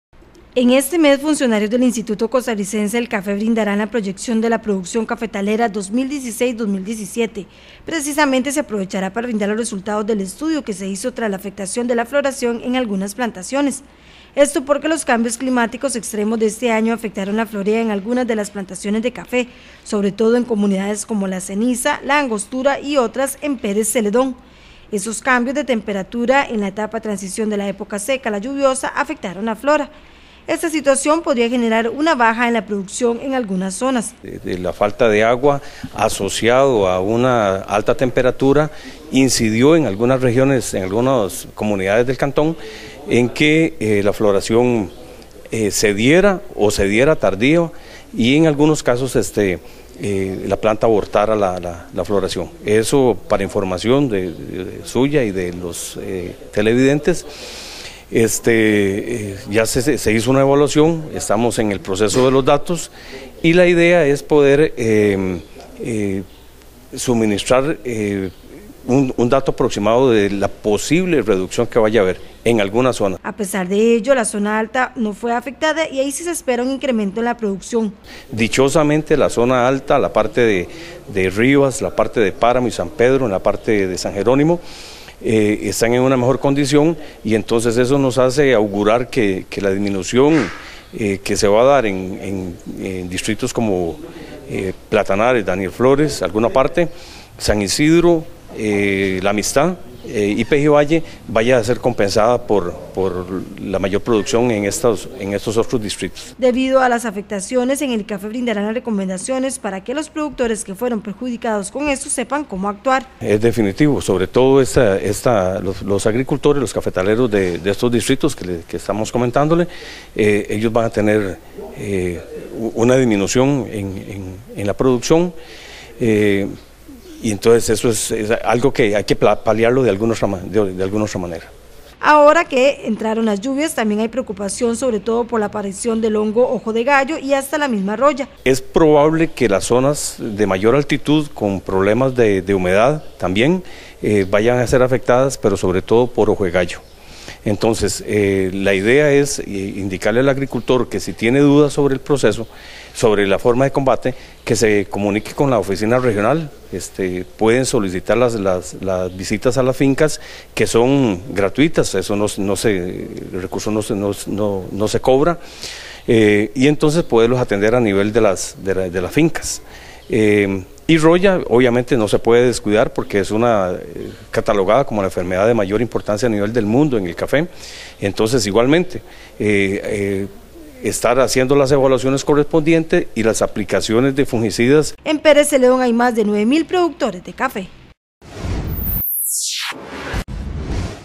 Producción cafetalera en Pérez Zeledón registraría una baja tras afectación en la floración
En este mes, funcionarios del Instituto Costarricense del Icafe, brindarán la proyección de la producción cafetalera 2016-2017.